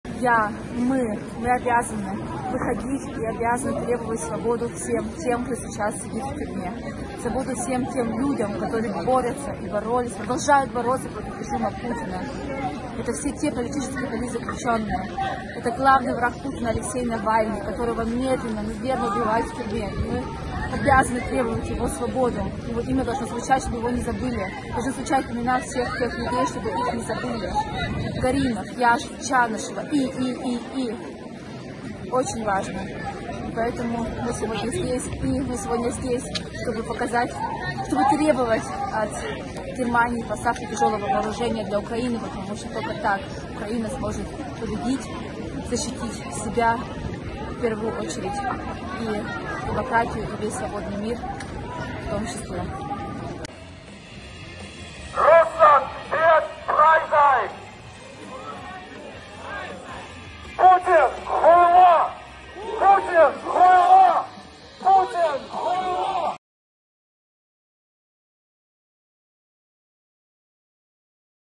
Митинг в Нюрнберге против Путина
Сегодня SOTA провела одиннадцатичасовой эфир и в течение целого дня показывала, как в разных городах и странах проходила всемирная акция в поддержку Алексея Навального и всех политзаключенных

Митинги приурочены к двухлетней годовщине возвращения политика в Россию. Активисты хотят показать политзаключенным, что они они не одни, и сказать власть, что репрессии не смогут подавить запрос общества на свободу.

Завтра SOTA вновь выйдет в спецэфир в 16.00. Смотрите его на нашем Youtube-канале.